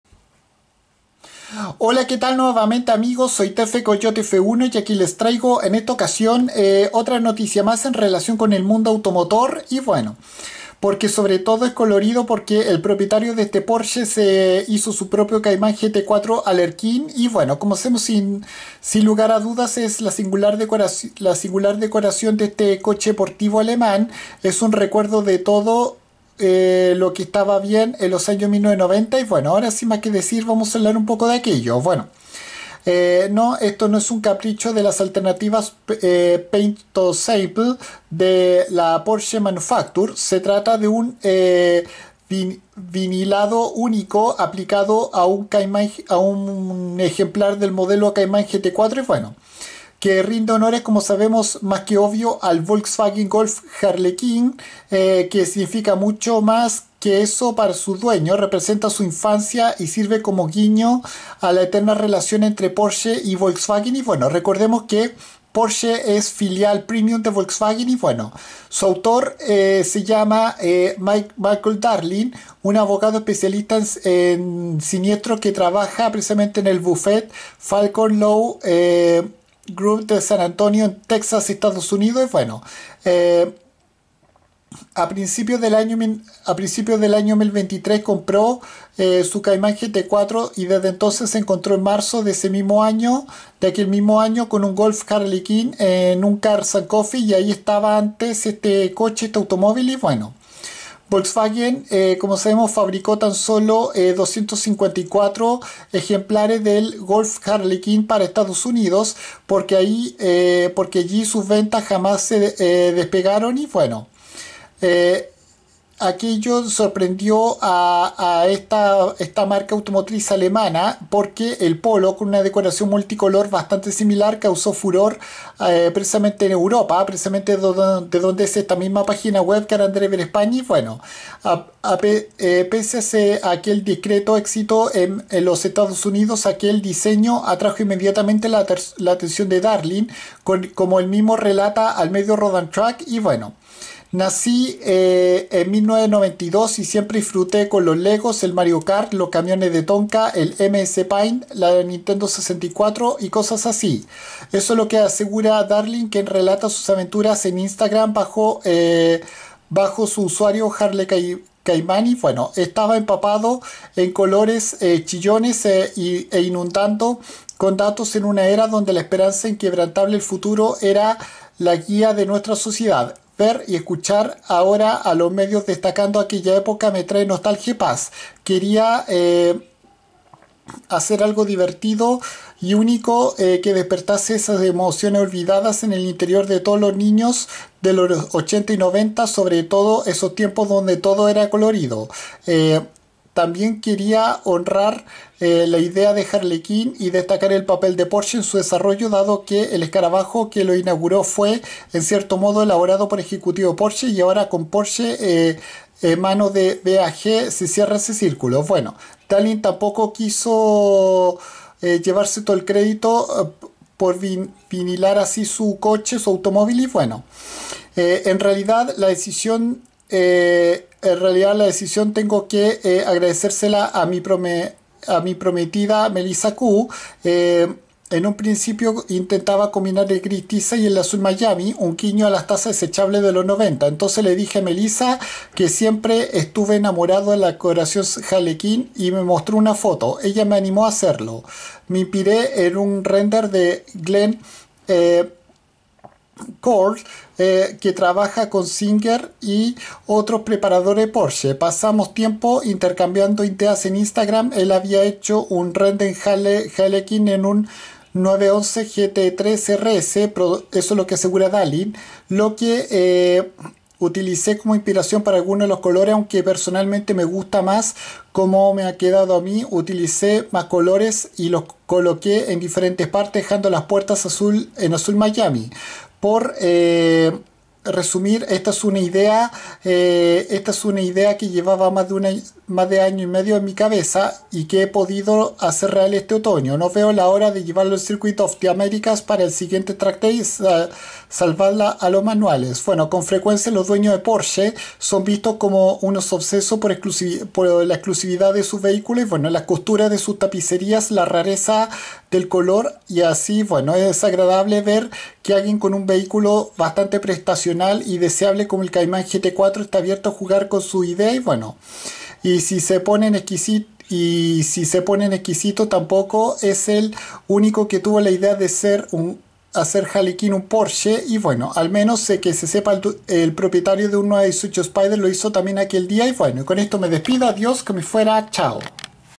El propietario de este Porsche Cayman GT4 lo convirtió en un Porsche "Alerquín"
Esta decoración hace referencia al raro VW Golf Alerquín que causó gran sensación en Europa en los años 1990